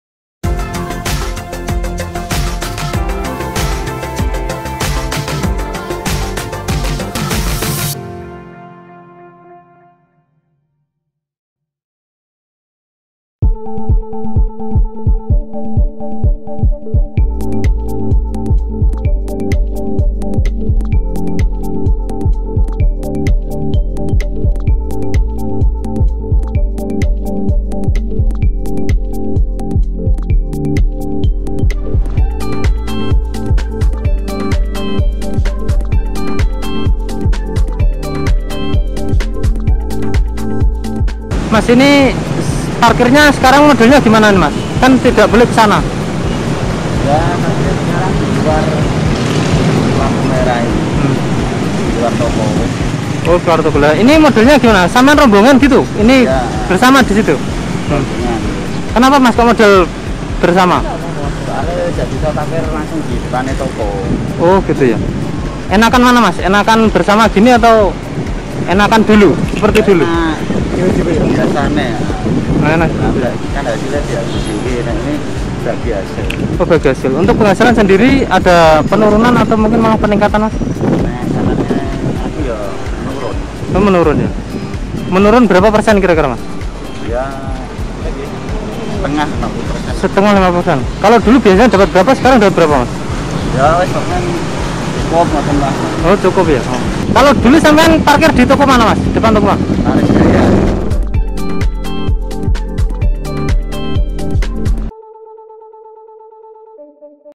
Imbas Penyekatan Jalan, Tukang Parkir Nyaris Kehilangan Pekerjaan
BETANEWS.ID, KUDUS - Di bawah terik Sang Surya, tampak dua pria beberapa kali meniup peluit yang menempel di mulutnya. Sembari mengarahkan beberapa pengendara yang akan parkir di samping titik penyekatan di Perempatan Traffic Light Pekojan, Jalan Sunan Kudus. Dua pria tersebut yakni para juru parkir yang mengaku hidupnya makin sulit sejak banyak penyekatan jalan di Kudus.

Adi, satu di antara juru parkir tersebut menuturkan, sejak ada Pemberlakuan Pembatasan Kegiatan Masyarakat (PPKM) Darurat se-Jawa dan Bali, di Kudus banyak penyekatan jalan. Tidak hanya di pintu masuk Kabupaten Kudus, tapi penutupan jalan juga terjadi di jalan - jalan perkotaan. Hal tersebut mengakibatkan penghasilannya turun drastis.